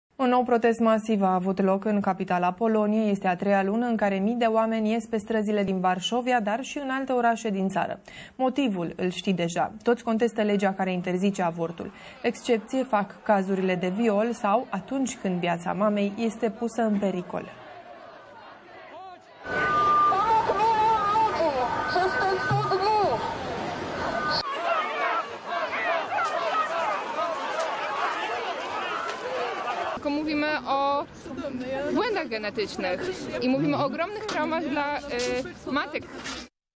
Proteste față de interzicerea avortului în Polonia
➥ Polonia se confruntă cu un nou val de proteste, în contextul înăspririi restricțiilor privind avortul;
➥ Realizarea avorturilor va fi posibilă doar în cazurile de viol, incest și în care viața sau sănătatea mamei era în pericol;
➥ Motivele prevăzute de lege au stat la baza a doar 2,4% din cele 1.100 de avorturi legale efectuate în Polonia în 2019;
➥ Autoritățile cer implementarea restricțiilor începând de azi;
➥ Protestele urmează să aibă loc și în alte orașe din țară, inclusiv în Cracovia, Lodz, Wroclaw și Poznan.